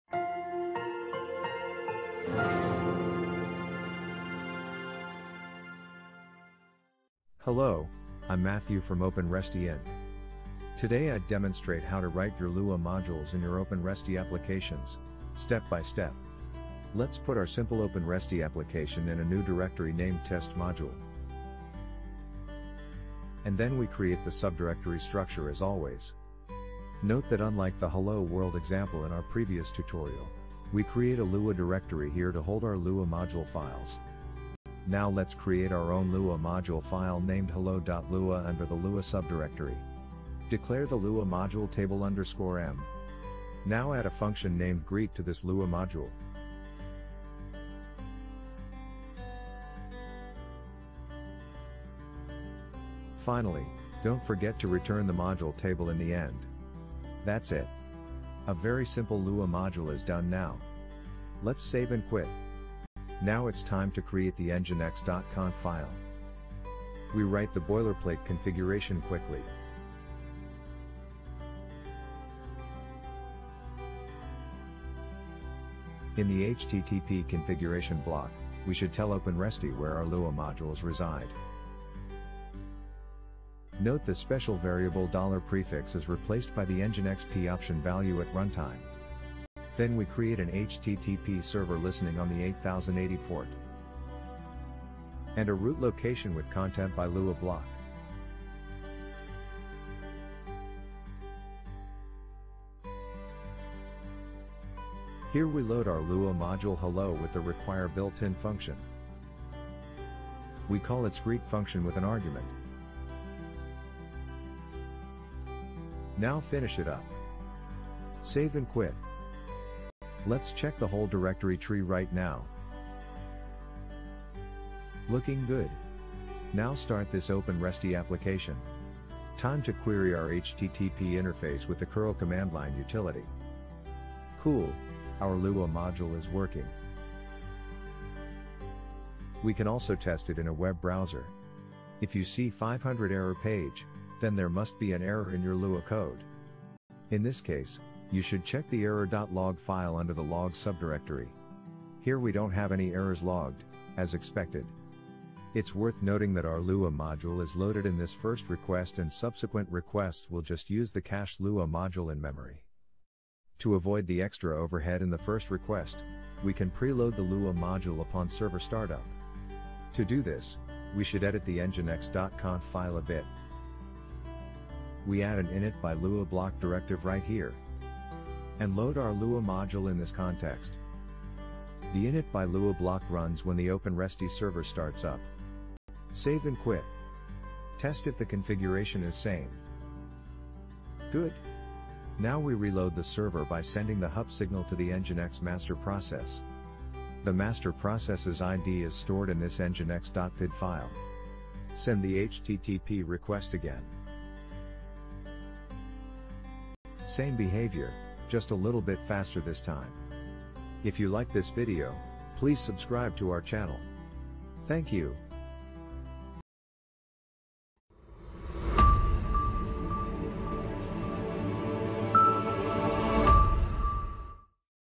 Write Your Own Lua Modules in OpenResty/Nginx Applications
This tutorial shows how to write your own Lua modules used in OpenResty applications, and how to preload Lua modules upon server startup. Text version of this tutorial